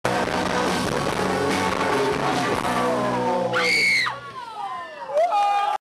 Kid Bombardos
Au BT59 le bassiste et ses pieds